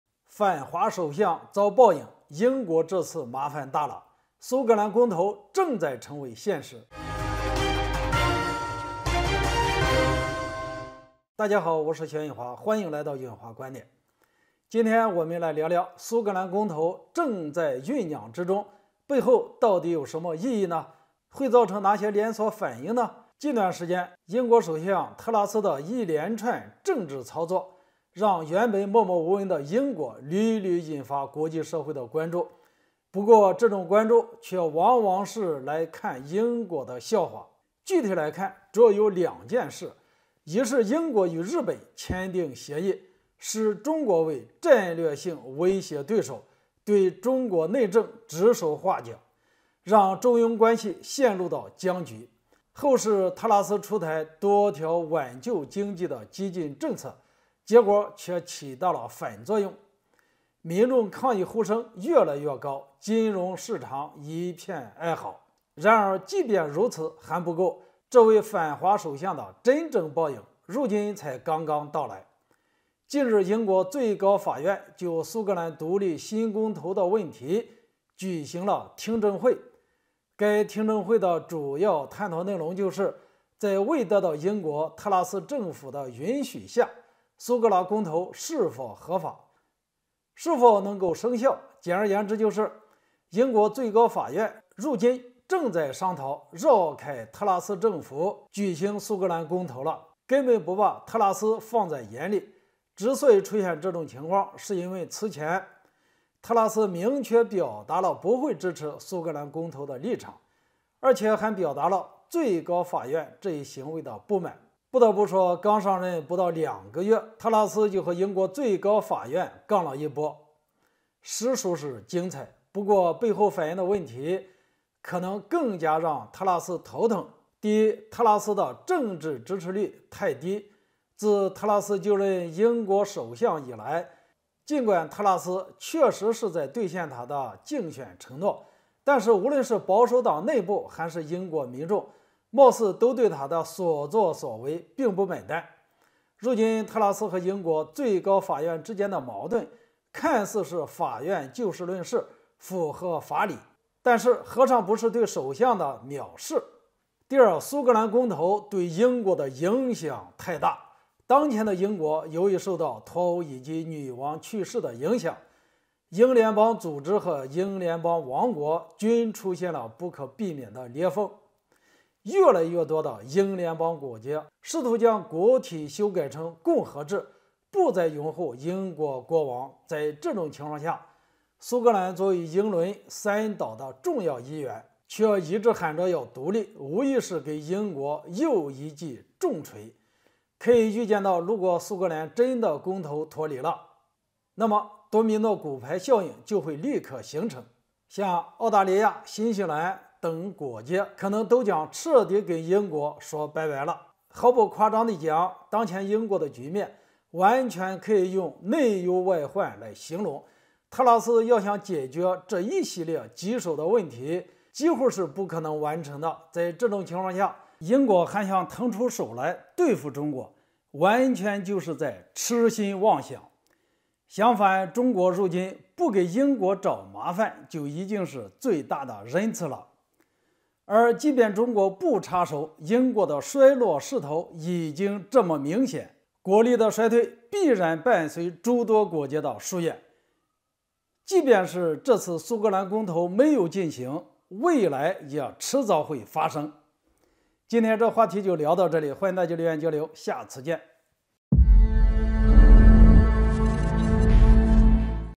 反華首相遭報應，英國這次麻煩大了，蘇格蘭公投正在成為現實【一號哨所】
反華首相遭報應，蘇格蘭公投正在醞釀中，背後到底有何意義？會造成哪些連鎖反應？
反華首相遭報應，英國這次麻煩大了，蘇格蘭公投正在成為現實